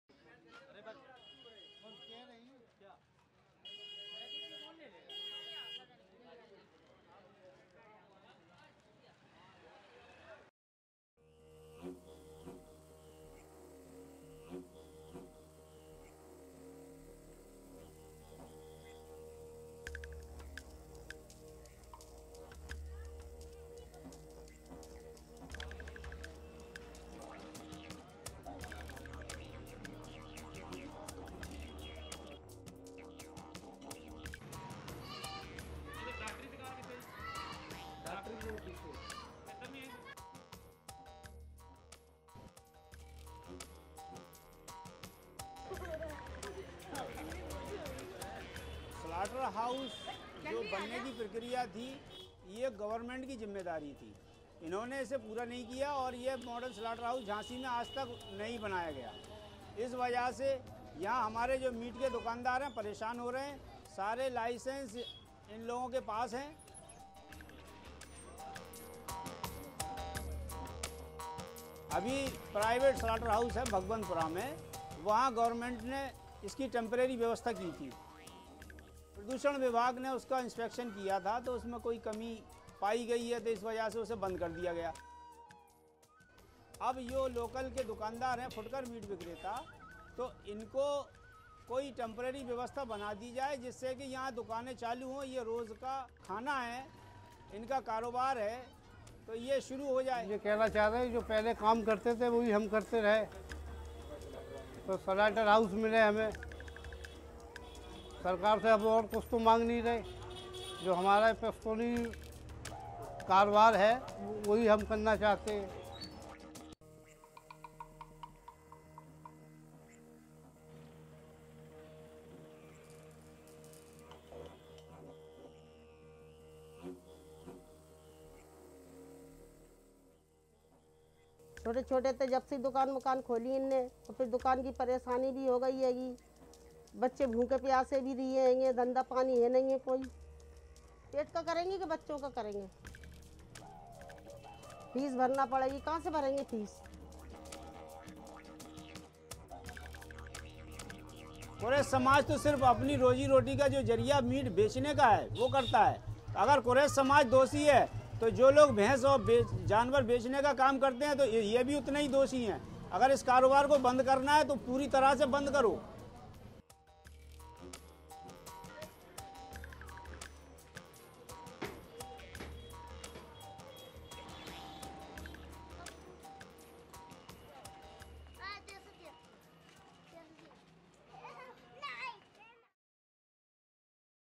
Why Meat Traders In UP Can’t Run Shops Even with Valid Licences
According to Indian law, meat traders cannot slaughter animals like buffaloes at their shops. They can only source meat from a slaughterhouse. But, with a crackdown on slaughterhouses, traders – mainly of the Quresh community – have no source of meat and their businesses are suffering.

In this video, Nafees Chaudhary, a social worker describes the plight of meat traders. Mohammad Noor, a meat trader shows proof of his valid licence and wishes to restart his business. Munni Qureshi, a homemaker says that it's getting difficult for the Quresh community to survive in the given circumstances.